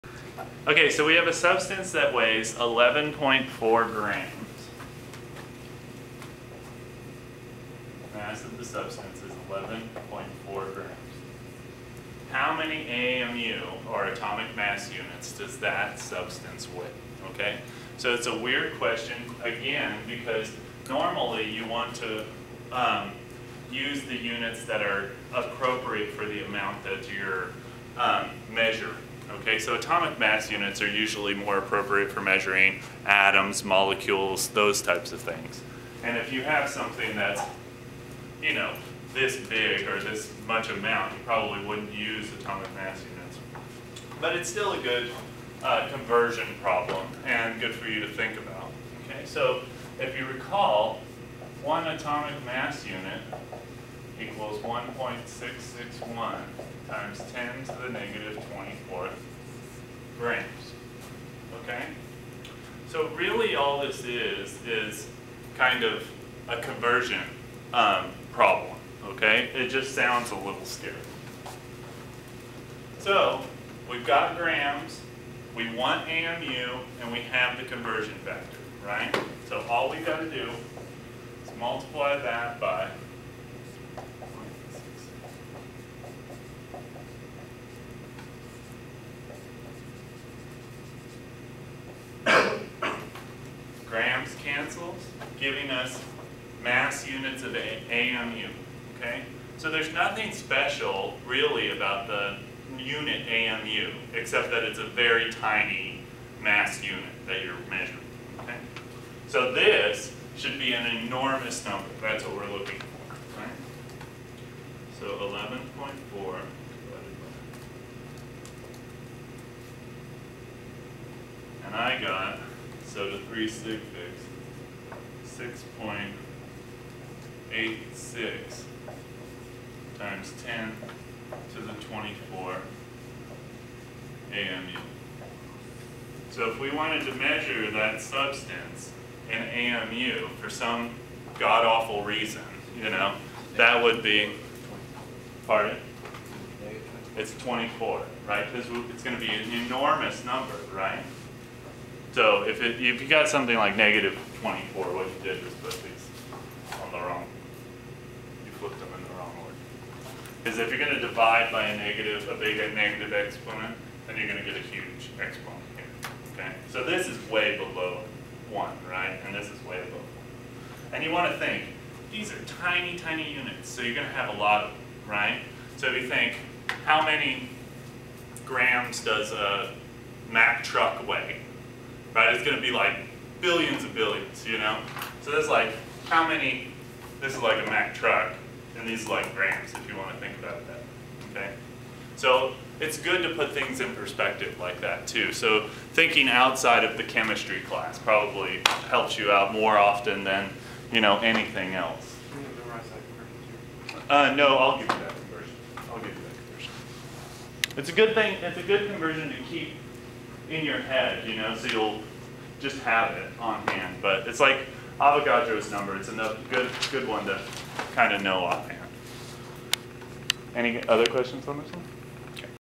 Convert Mass to Mass - Atomic Mass Units to Grams
A substance weighs 11.4 g, how many amu does this substance weighs?

Interviews
1) Revell, K. (November 16, 2016) “An Interview with Heath Giesbrecht, Part I” The Macmillan Community

2) Revell, K. (November 16, 2016) “An Interview with Heath Giesbrecht, Part II” The Macmillan Community